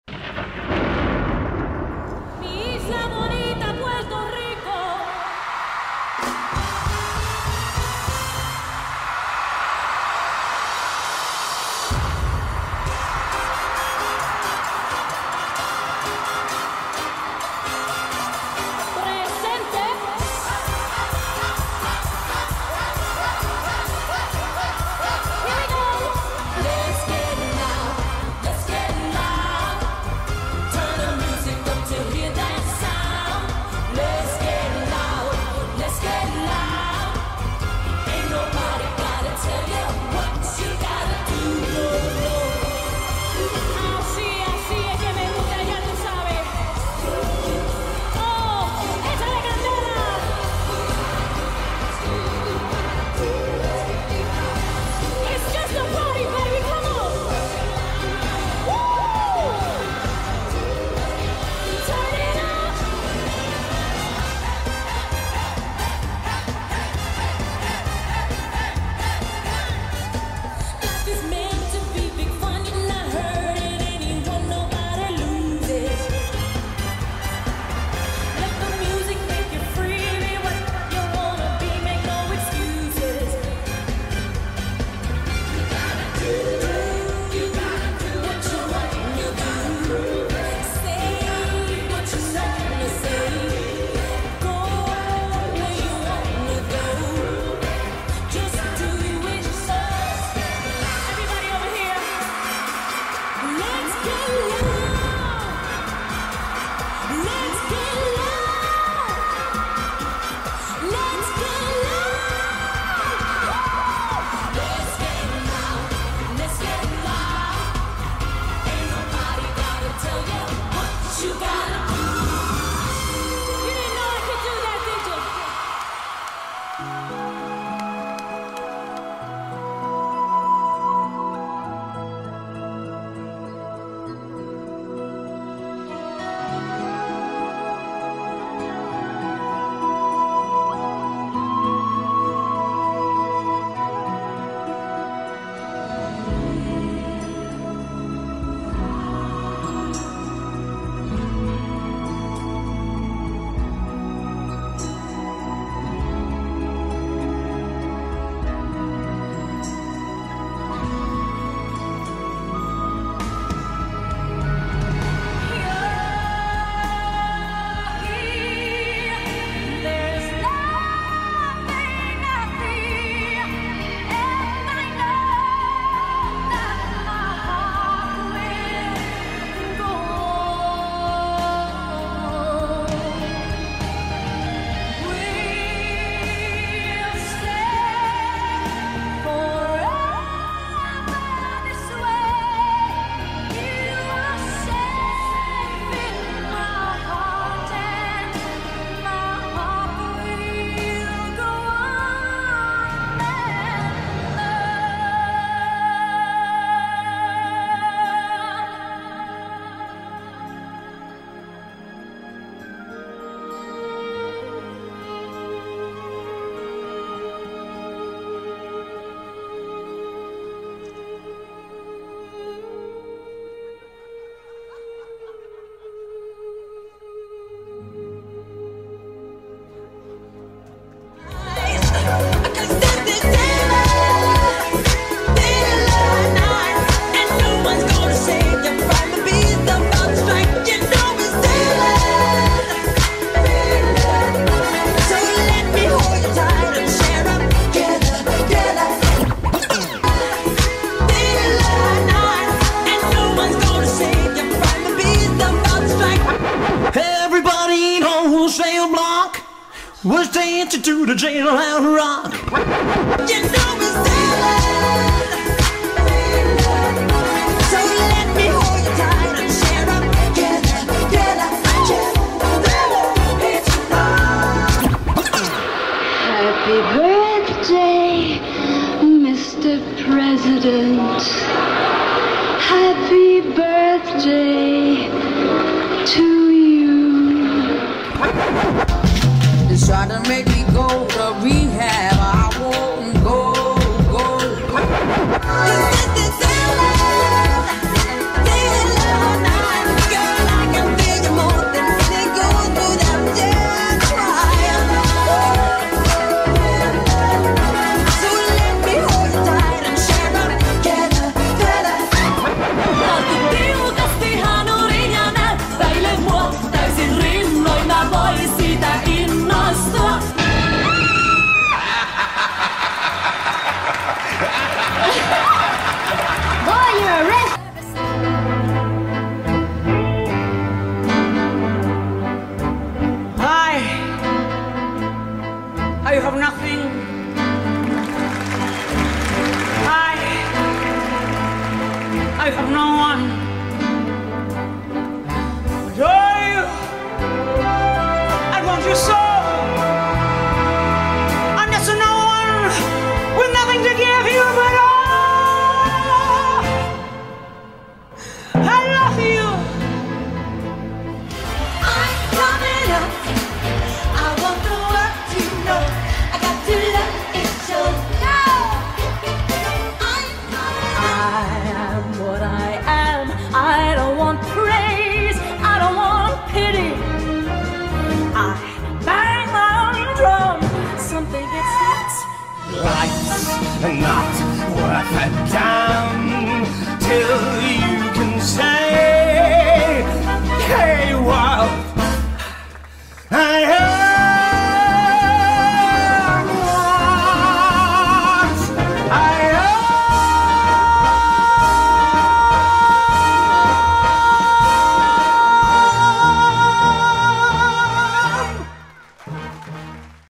Divet Show by Marko Vainio Silja Serenade/ Grand Opening 2012
Suomen suosituin ja hauskin dragshow! Divet Show by Marko Vainio tuo yli kolmekymmentä kansainvälistä ja kotimaista artistia lavalle! Tilaa tähdet tilaisuuteesi!

Mitä pidit videosta? Kommentoi alle

Divet Show by Marko Vainio, an award-winning celebrity impersonator and drag superstar from Finland!